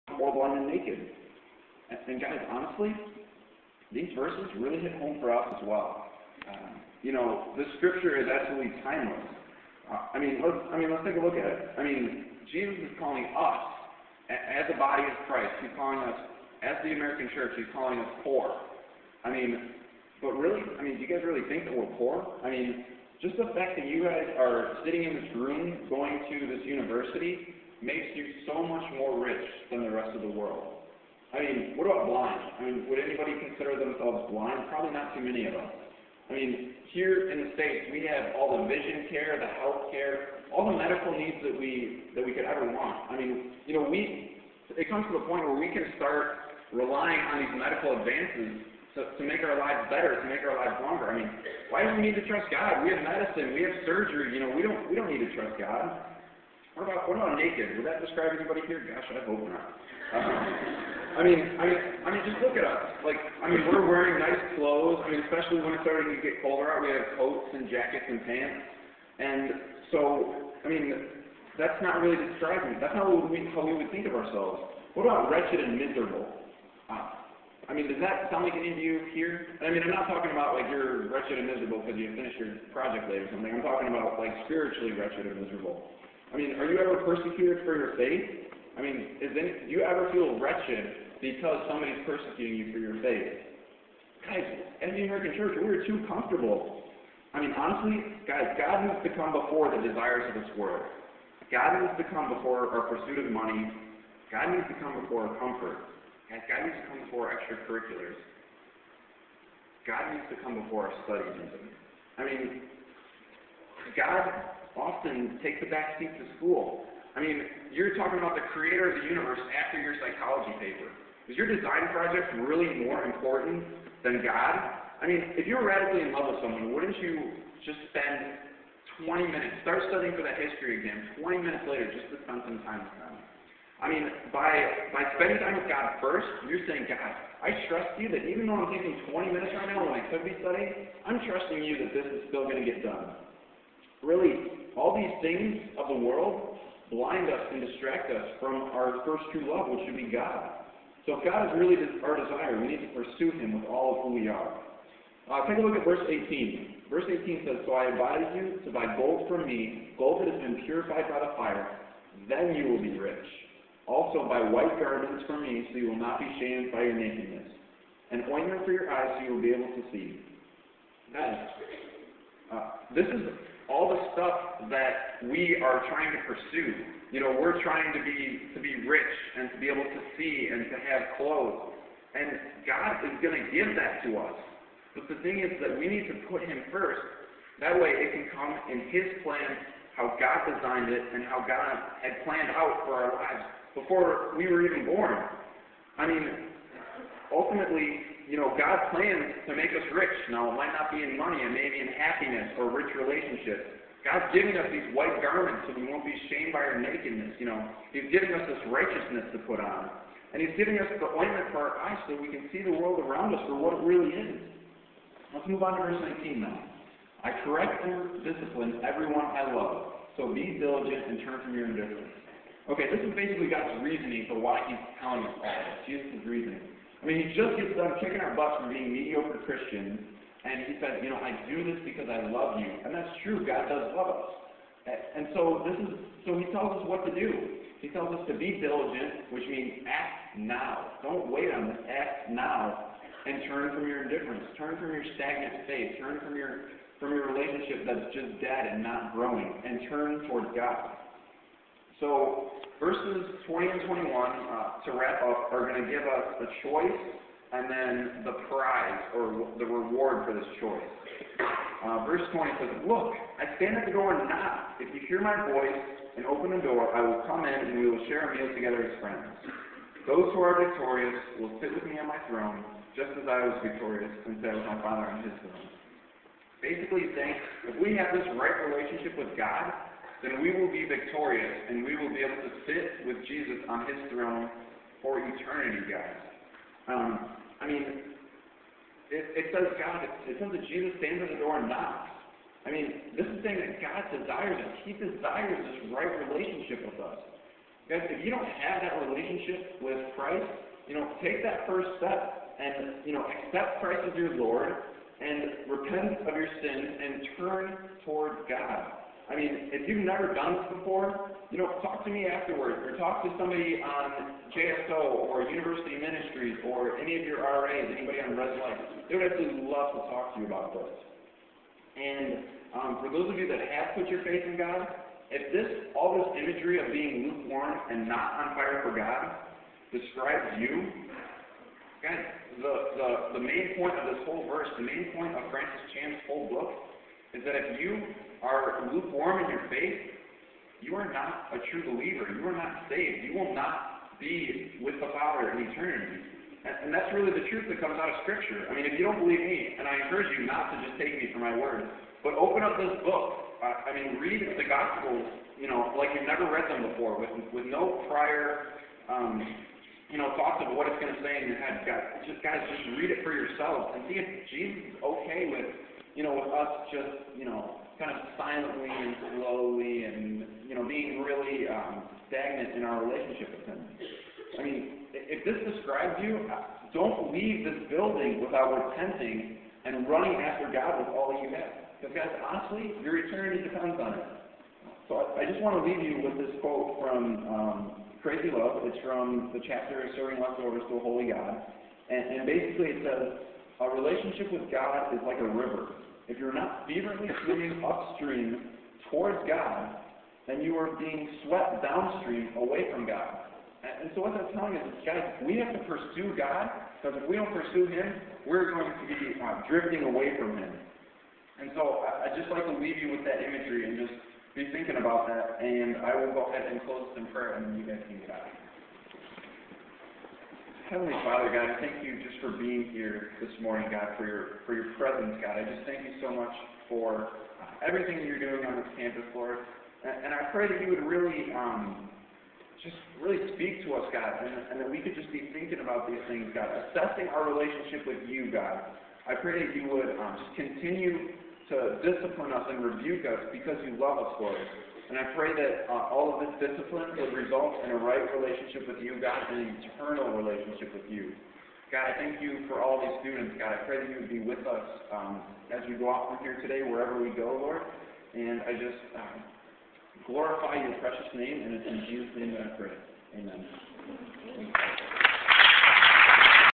Judson University Chapel - Ryan Reopelle - November 9, 2009 - Part 4
Part 4: Judson University men's soccer player Ryan Reopelle talks about the book Crazy Love by Francis Chan.  He speaks on one chapter out of the book.